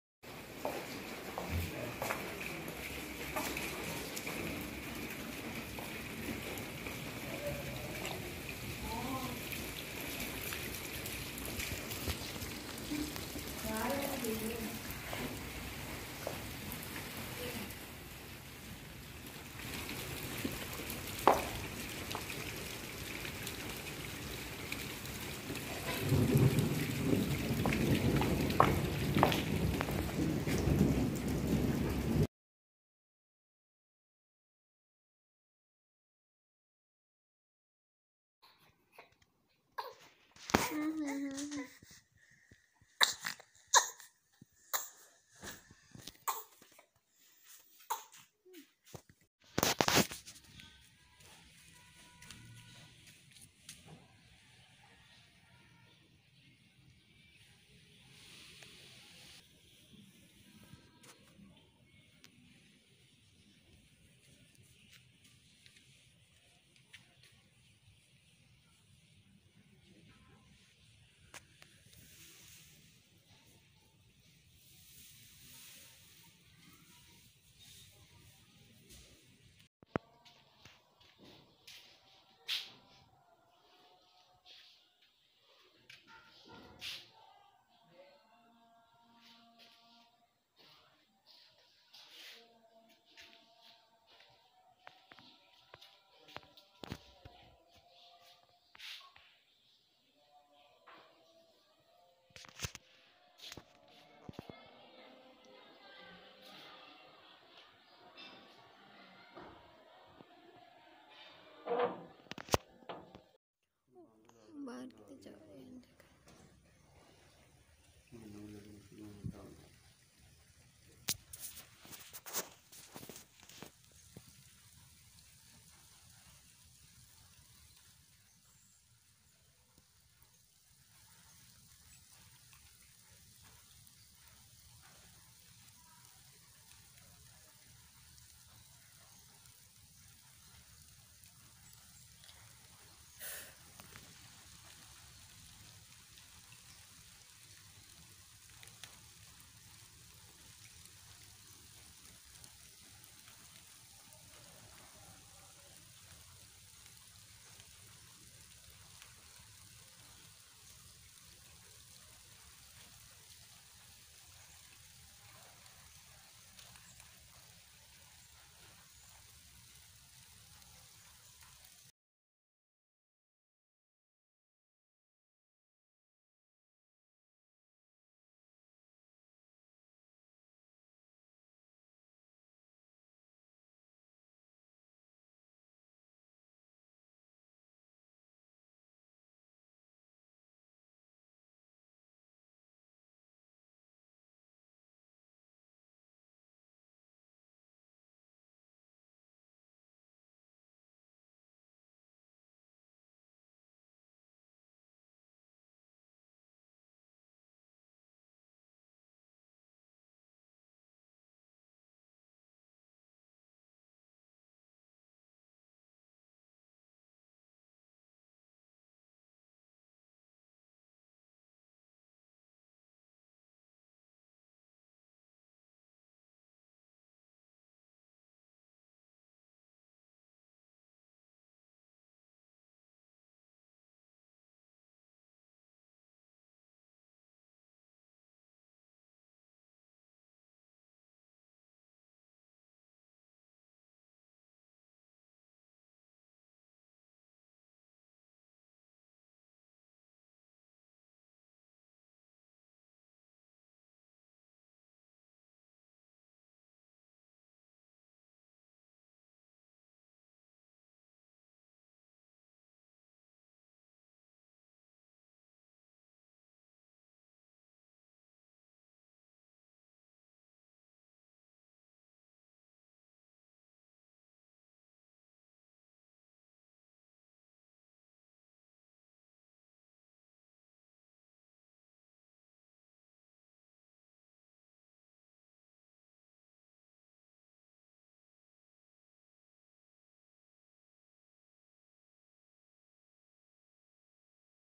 Rainy days 🌧🌧🌧🌧
rainy days all around water and very bad situation 🙁rain